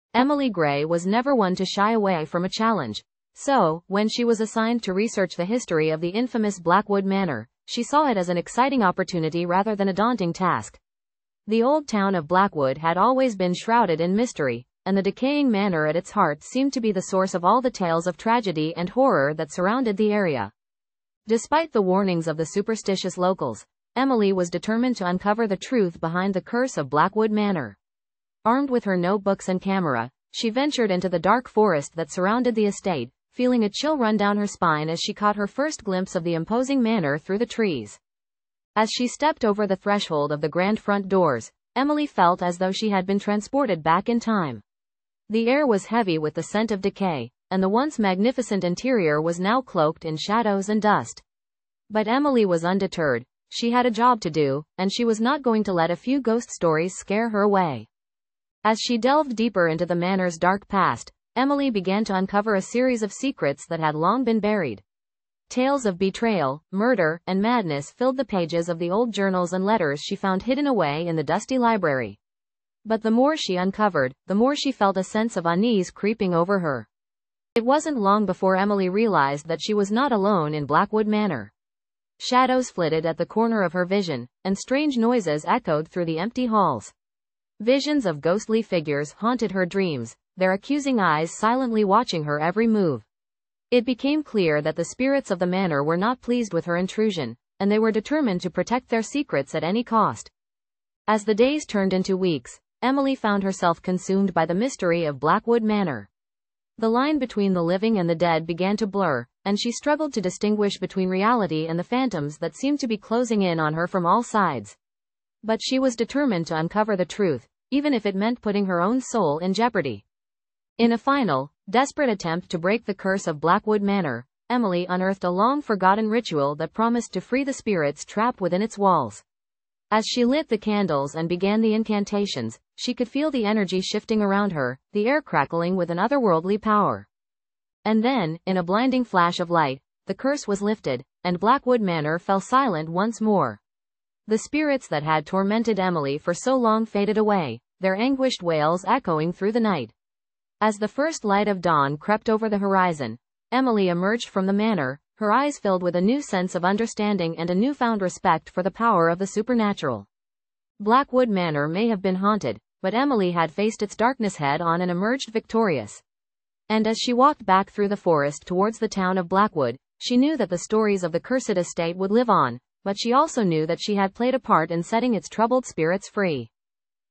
Haunted Secrets of Blackwood Manor: The Dark Truth Revealed易
Title: The Haunting of Blackwood Manor

In the old town of Blackwood, nestled deep within the dark forest, lies the decaying manor of the Blackwood family. For generations, whispers of tragedy and horror have surrounded the cursed estate, with locals avoiding it like the plague.

When young historian, Emily Grey, is tasked with researching the history of Blackwood Manor, she is undeterred by the warnings of the townspeople. Ignoring their ominous warnings, she ventures into the overgrown grounds and unlocks the grand front doors, stepping into a world frozen in time.

As Emily delves deeper into the manor's dark past, she uncovers a web of secrets and lies that have long been buried. But the spirits that dwell within the walls of the manor are not pleased with her intrusion, and they are determined to ensure that the truth remains hidden at any cost.

Haunted by malevolent forces and plagued by visions of the past, Emily must unravel the mystery of Blackwood Manor before it consumes her soul. But as she delves deeper into the darkness, she soon realizes that some secrets are better left buried, for the dead do not take kindly to those who disturb their eternal rest.

Will Emily uncover the truth behind the curse of Blackwood Manor, or will she become another victim of its haunting legacy?